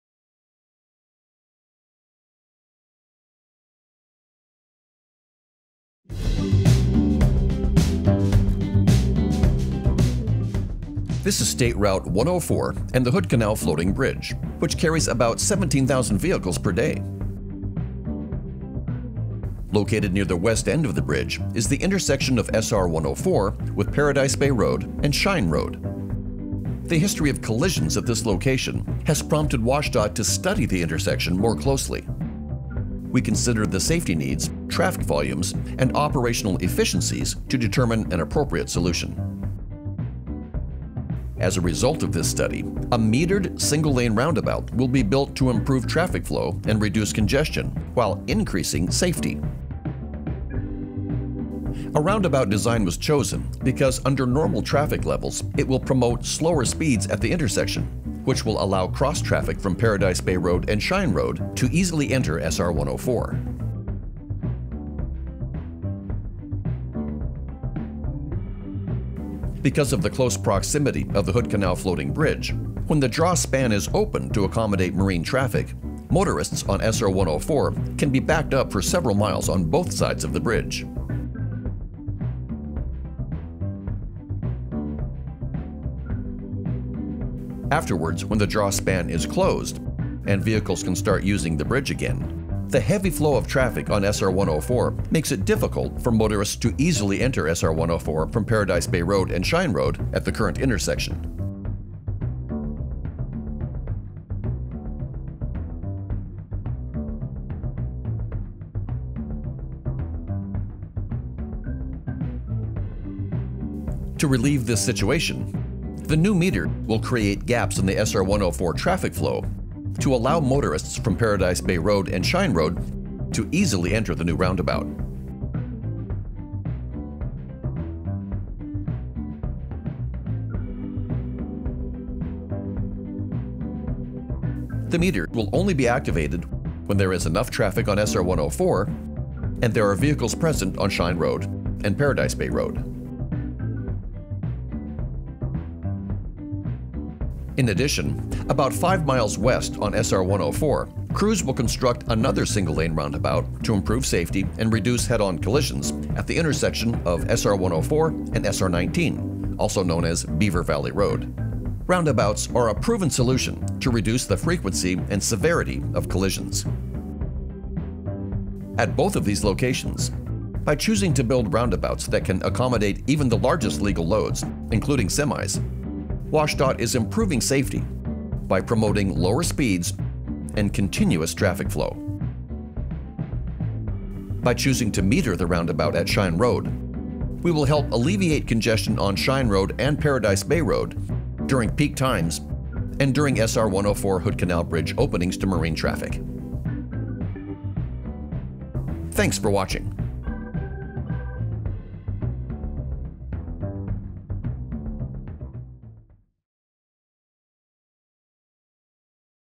SR 104 at Shine Road/Paradise Bay Road and SR 19 - Intersection Safety Improvement Project
The intersection of State Route 104 and Shine Road/Paradise Bay in Jefferson County near the Hood Canal Bridge has a history of serious-injury collisions. In early 2019, we conducted a formal analysis called an Intersection Control Evaluation which considered safety needs, operational efficiencies and area traffic volumes with the goal of identifying improvements for the traveling public. The study recommendation was to build a single-lane, metered roundabout at the intersection to reduce the probability of collisions and improve traffic flow. This project will build roundabouts on SR 104 at both Shine Road/Paradise Bay Road and at SR 19/Beaver Valley Road. Both projects are anticipated to begin construction in 2024. Keep up to date with construction on our project